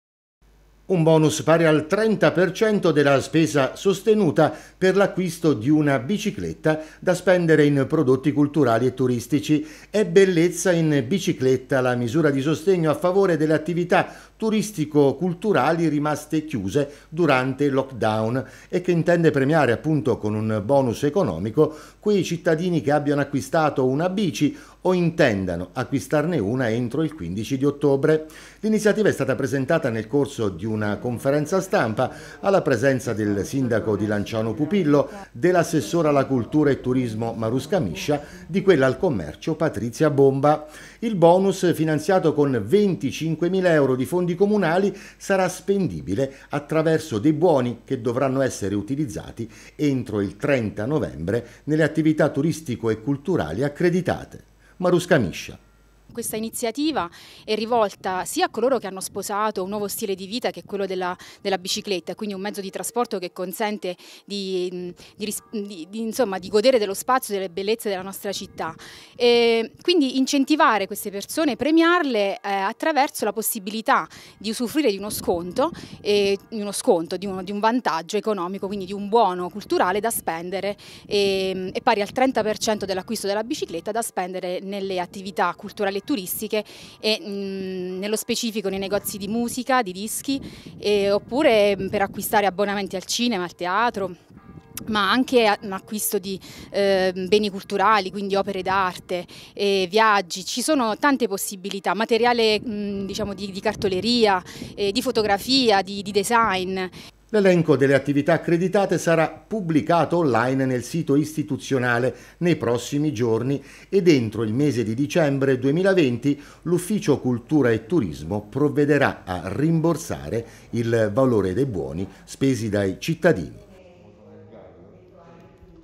Lanciano promuove "Bellezza in bicicletta"
Lanciano promuove "Bellezza in bicicletta": l'acquisto di una bicicletta verrà premiato con un bonus di massimo 300 euro da spendere nel settore cultura e turismo per aiutare i settori maggiormente penalizzati dalla crisi economica a seguito del lockdown.